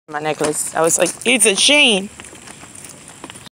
Chihuahua & baby in a stroller
Walking ny baby & Chihuahua at the same time lol!